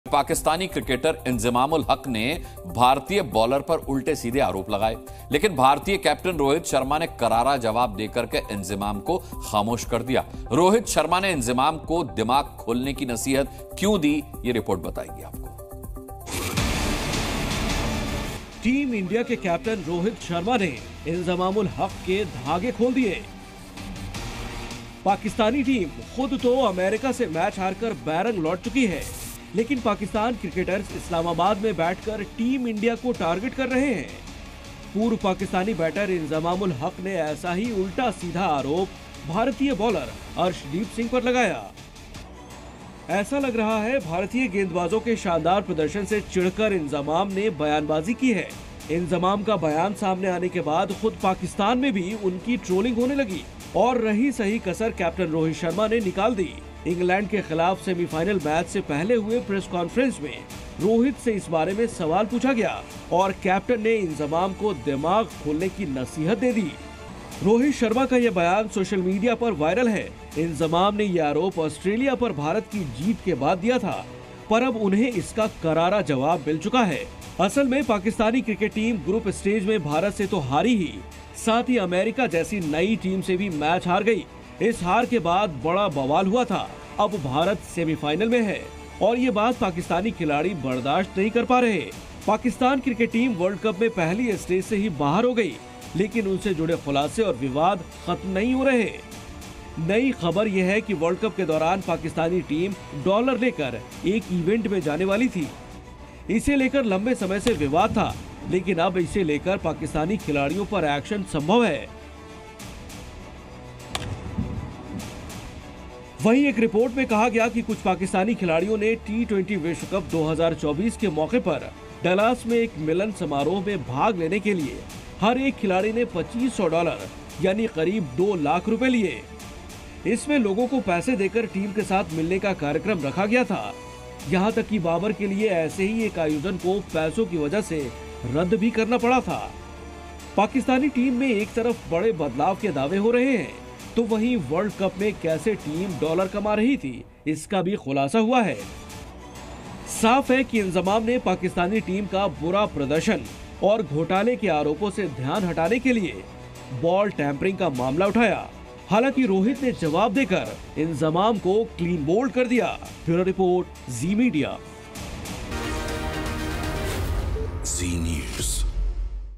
IND vs ENG Semi Final: फंसा सेमीफाइनल..भारत ने पाक के धागे खोल दिए! | T20 World Cup | Breaking News
IND vs ENG Semi Final T20 World Cup 2024: भारत-इंग्लैंड के बीच गुयाना में सेमीफाइनल चल रहा है. बारिश के चलते मैच रद्द होने की संभावना है.वर्ल्डकप में भारतीय क्रिकेट टीम को सेमीफाइनल में देखकर पाकिस्तान जल रहा है.इस कोशिश में पाकिस्तानी क्रिकेटर इंजमाम उल हक ने भारतीय बॉलर पर उल्टे सीधे आरोप लगाये लेकिन भारतीय कैप्टन रोहित शर्मा ने करारा जवाब देकर इंजमाम को खामोश कर दिया.

IND vs ENG Semi Final T20 World Cup 2024: Semi-final is going on between India and England in Guyana. There is a possibility of the match being canceled due to rain. Pakistan is jealous after seeing the Indian cricket team in the semi-finals of the World Cup. In this attempt, Pakistani cricketer Inzamam ul Haq made direct allegations against the Indian bowler but Indian captain Rohit Sharma gave a befitting reply to Inzamam.